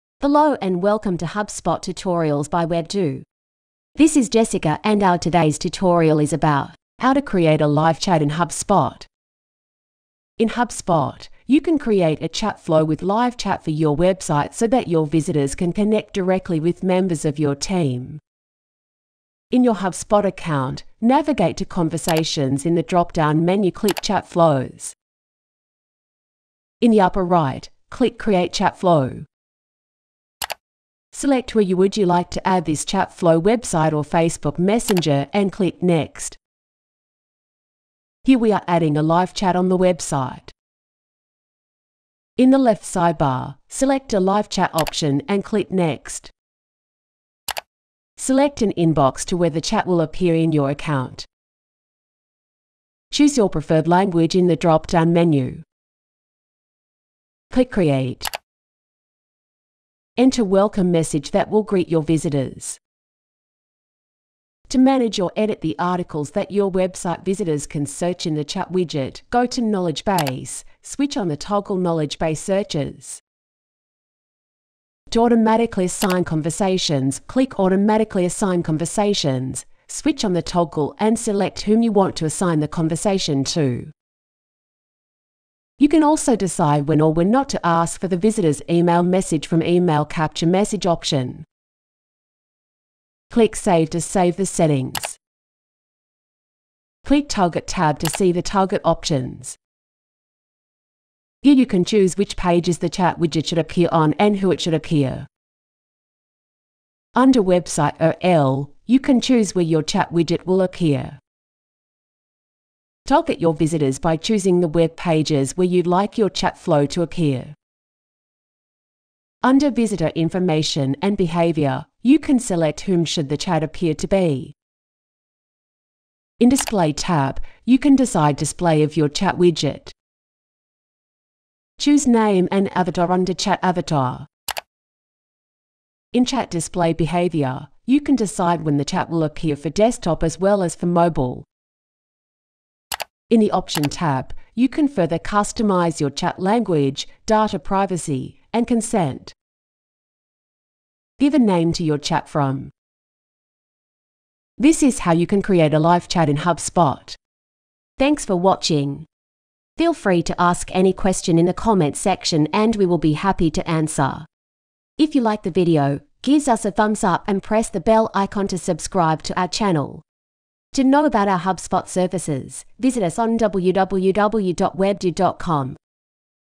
How-to create a live chat in HubSpot
In this HubSpot tutorial, we will walk you through how-to create a live chat in HubSpot.

In HubSpot, you can create a chat flow with live chat for your website so that your visitors can connect directly with members of your team.

Steps to follow:

1. In your HubSpot account, navigate to Conversations in the dropdown menu click Chat Flows.

2. In the upper right, click Create chat flow.

3. Select where you would you like to add this chat flow website or Facebook messenger and click next

4. Here we are adding a live chat on the website.

5. In the left sidebar, select a live chat option and click next.

6. Select an inbox to where the chat will appear in your account.

7. Choose your preferred language in the dropdown menu.

8. Click Create.

9. Enter a welcome message that will greet your visitors.

10. To manage or edit the articles that your website visitors can search in the chat widget, go to Knowledge Base, and switch on the toggle Knowledge base searches.

11. To automatically assign conversations, click automatically assign conversations, switch on the toggle and select whom you want to assign the conversation to.

12. You can also decide when or when not to ask for the visitor’s email message from the Email capture message option.

13. Click Save to save the settings.

14. Click the target tab to see the target options.

15. Here you can choose which pages the chat widget should appear on and to who it should appear.

16. Under the Website URL, you can choose where your chat widget will appear.

17. Target your visitors by choosing the web pages where you’d like your chat flow to appear.

18. Under visitor information and behavior, you can select whom should the chat appear to be.

19. In the display tab, you can decide to display your chat widget.

20. Choose name and avatar under Chat avatar.

21. In chat display behavior, you can decide when the chat will appear for desktop as well as for mobile.

22. In the Option tab, you can further customize your chat language, data privacy, and consent.

This is how you can create a live chat in HubSpot.